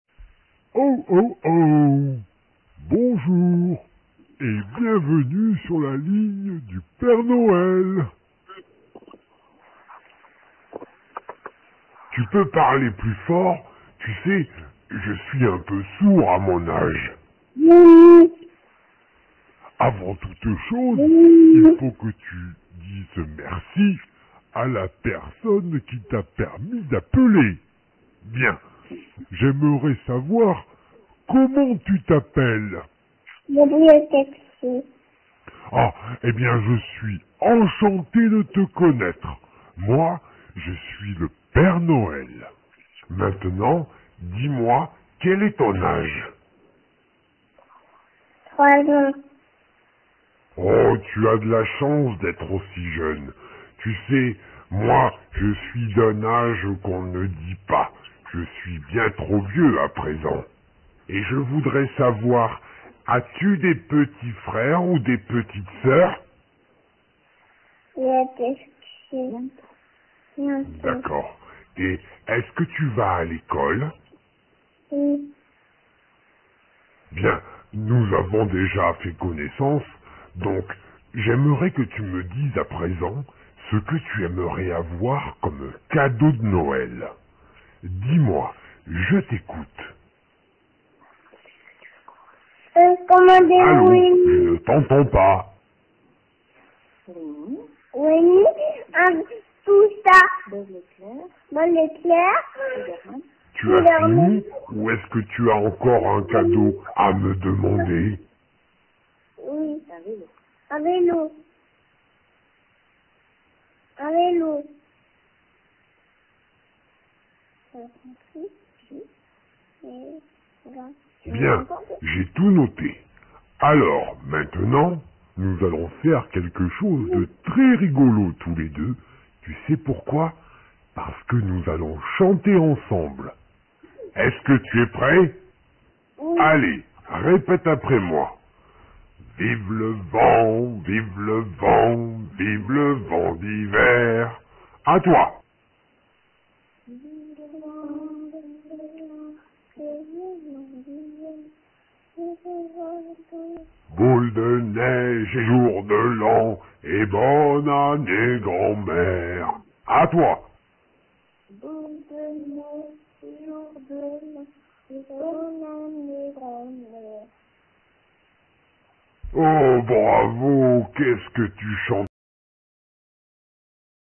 Gabriel appelle le téléphone du Père Noël et parle avec lui en personne le 19-12-2014 a 18:
Gabriel a donné sa liste de cadeaux au Père Noël Regardez sa vidéo souvenir de ce moment magique !